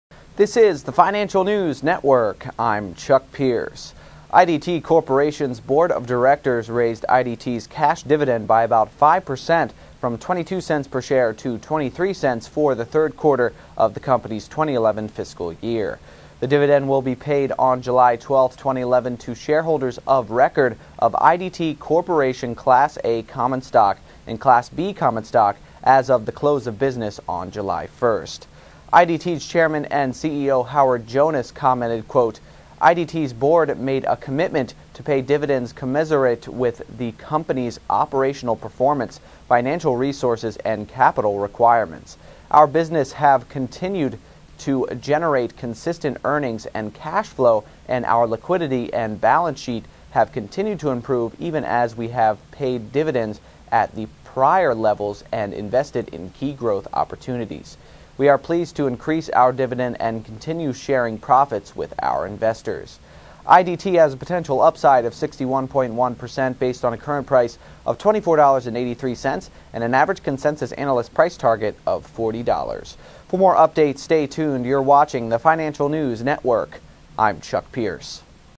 IDT Corp's Board Raised Its Cash Dividend To $0.23 For Q3
IDT Corporation's (NYSE:IDT) board of directors raised IDT's cash dividend by about 5% from $0.22 per share to $0.23 for Q3 of the company's 2011 fiscal year. The dividend will be paid on July 12, 2011 to shareholders of record of IDT Corporation Class A Common Stock and Class B Common Stock as of the close of business on July 1st.

IDT's Chairman and CEO, Howard Jonas commented, "IDT's Board made a commitment to pay dividends commensurate with the Company's operational performance, financial resources and capital requirements. Our businesses have continued to generate consistent earnings and cash-flow and our liquidity and balance sheet have continued to improve even as we have paid dividends at the prior levels and invested in key growth opportunities. We are pleased to increase our dividend and continue sharing profits with our investors."

IDT has a potential upside of 61.1% based on a current price of $24.83 and an average consensus analyst price target of $40.